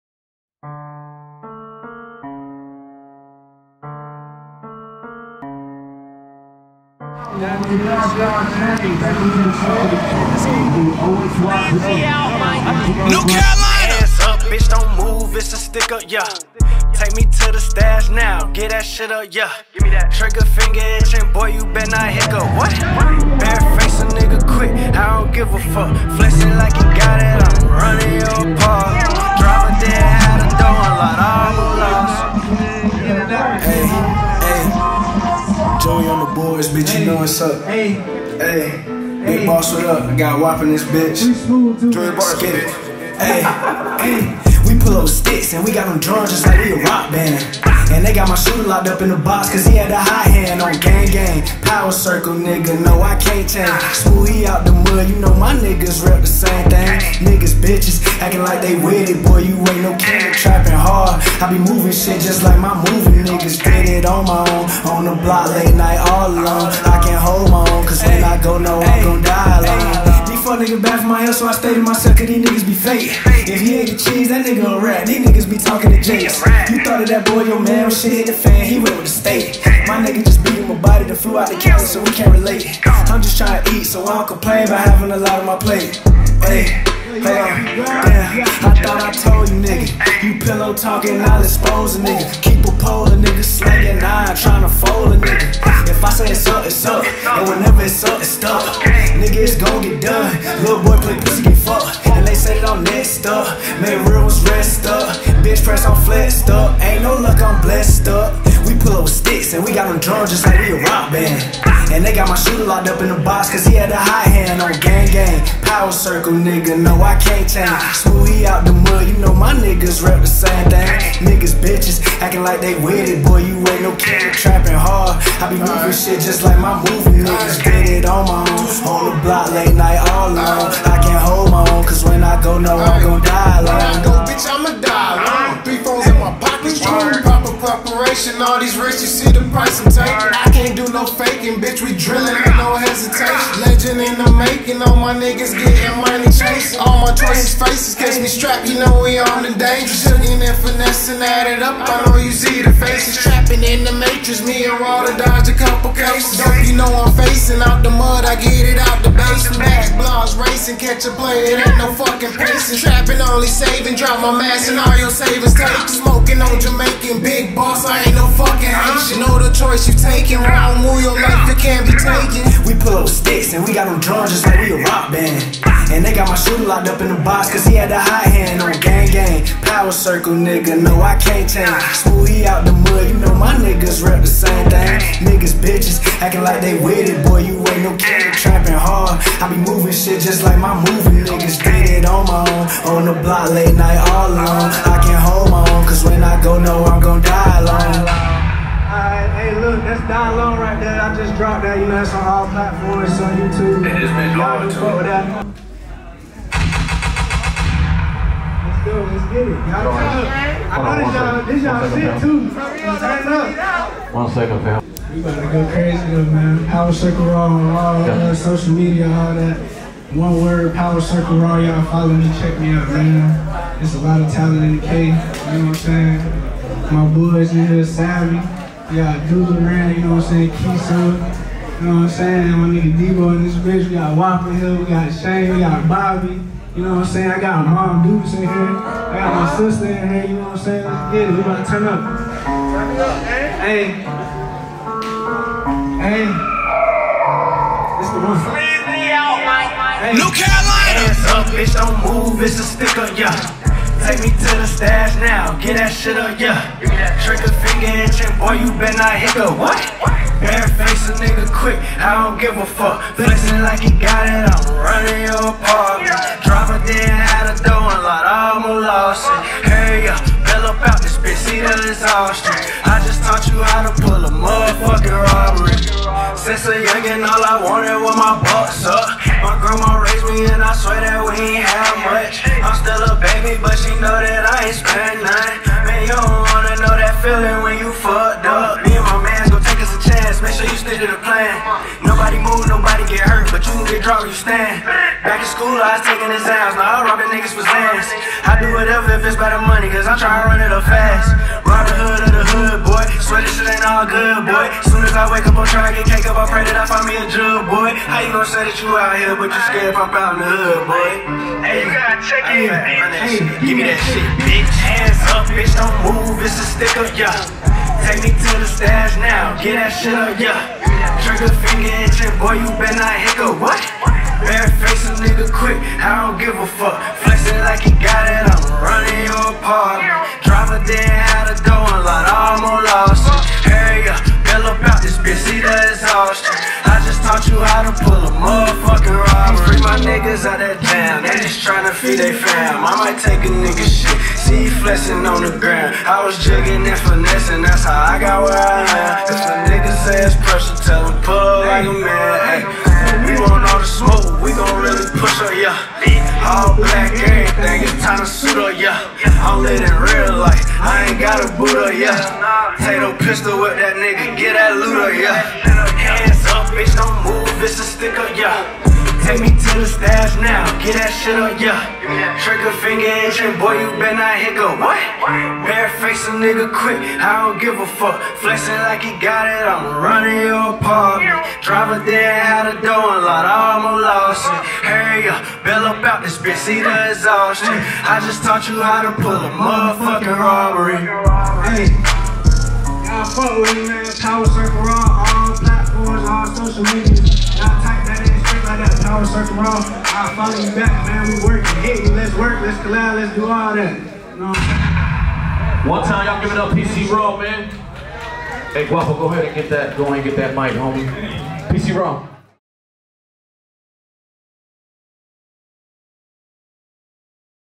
Power Circle Raw VLOG @ I LOVE NEW MUSIC INDIE FESTIVAL KINSTON NC
PERFORMANCE FOOTAGE FROM THE I LOVE NEW MUSIC INDIE FESTIVAL HELD IN KINSTON NC

FOLLOW @POWERCIRCLERAW AND @JEFFKANE000